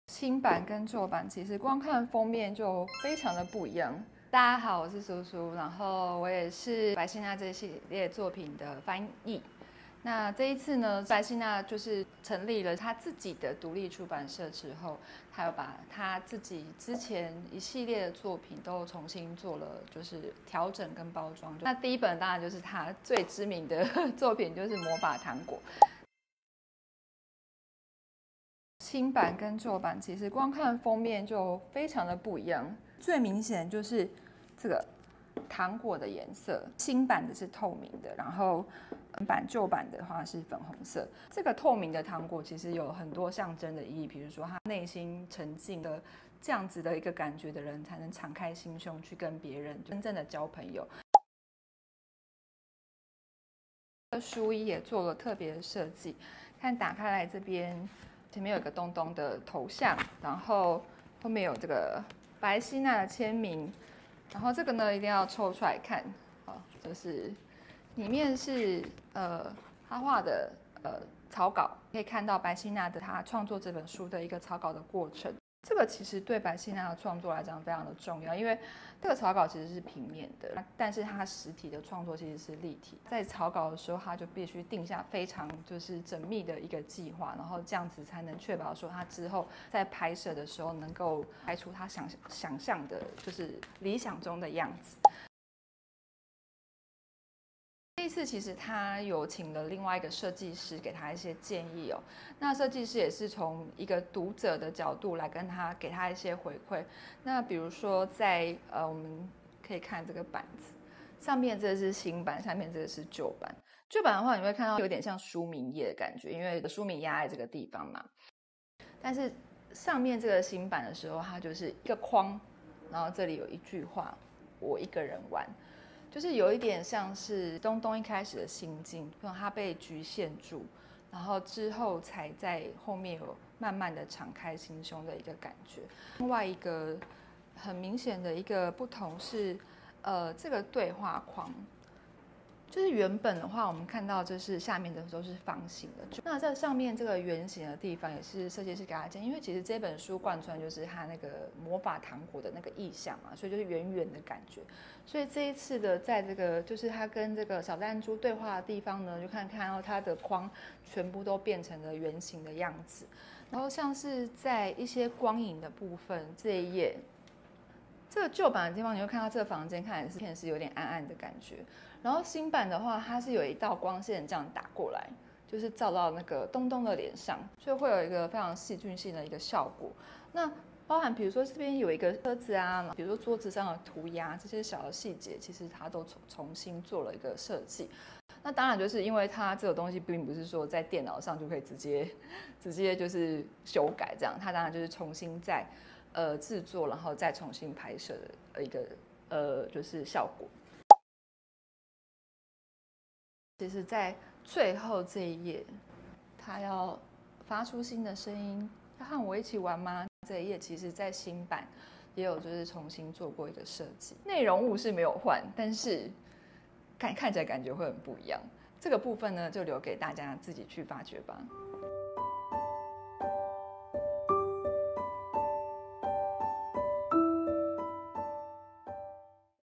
【維京國際】嶄新亮相！韓國繪本作家白希那《魔法糖果》「暢銷典藏版」✨
韓國超人氣創作者 白希那繪本
──《魔法糖果》全新升級改版
很榮幸邀請本書譯者蘇蘇老師
陪我們揭開「暢銷典藏版」的祕密

🍬《魔法糖果（暢銷典藏版）》
☆首批加贈A5「魔法資料夾」☆

文/圖：白希那

𝓢𝓽𝓸𝓻𝔂

小男孩東東總是一個人玩，
他想著，他們都不和我玩，
我一個人也能玩得很開心！
但東東真的喜歡一個人玩嗎？

直到有天，他得到了一些特別的糖果，
上面的花色好像有點似曾相似
此時，耳邊傳來奇怪的聲音.

白心那暖心治癒的繪本佳作
用心傾聽，會發現自己從不孤單，
愛，一直都在……